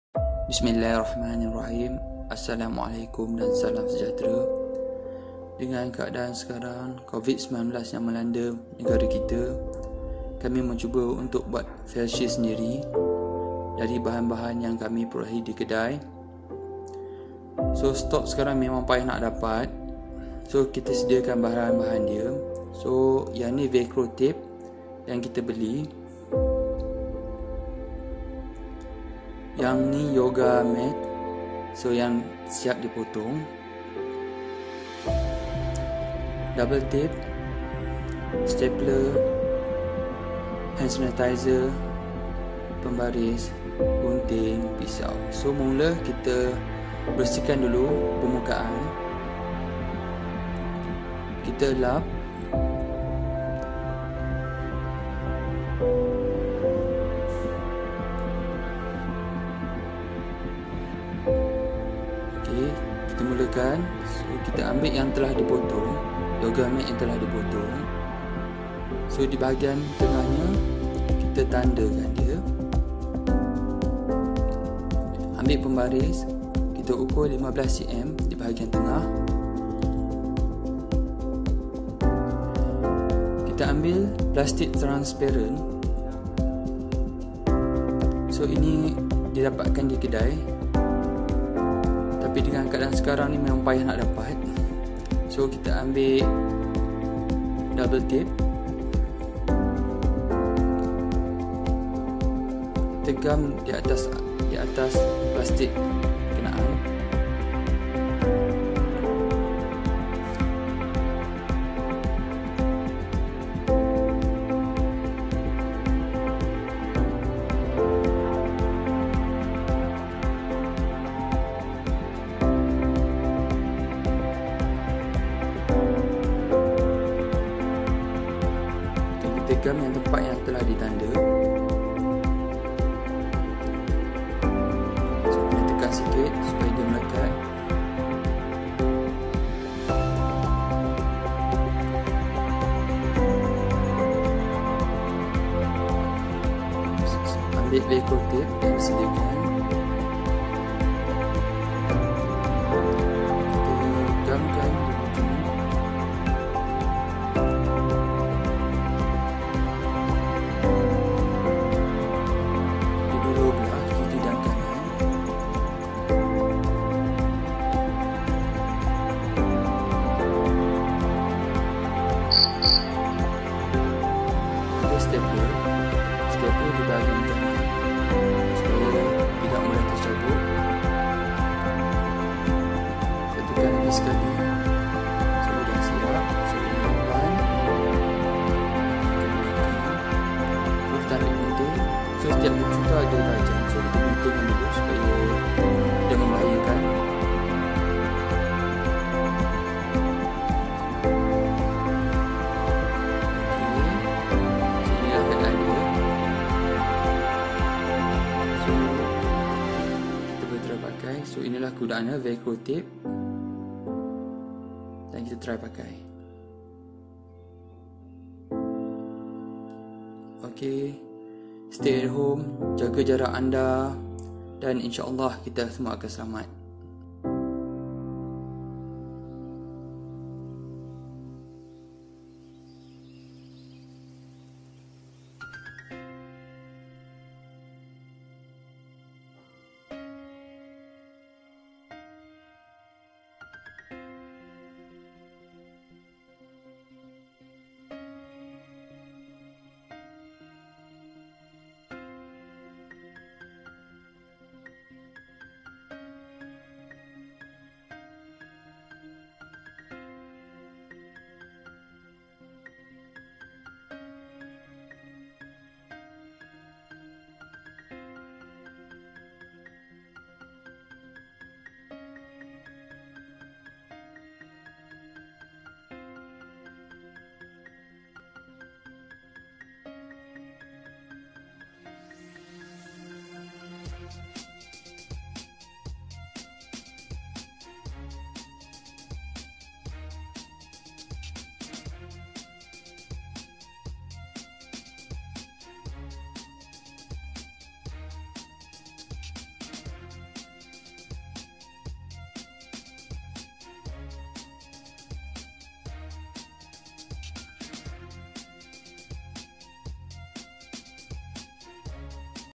DIY Face Shield Pusat Kesihatan Universiti UPM
Saksikan inisiatif Pusat Kesihatan Universiti UPM membuat face shield secara DIY untuk digunakan oleh petugas-petugas menyaring pesakit-pesakit Covid-19.